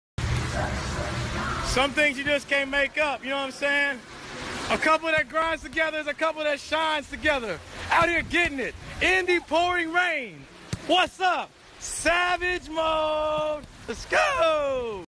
'The couple that grinds together' Jordin Sparks beau work out @ NEWSXPS
'The couple that grinds together, shines together': Jordin Sparks & beau work out in rain. Dana Isaiah is a model and fitness enthusiast and despite the gray weather and falling rain, it didn't stop the two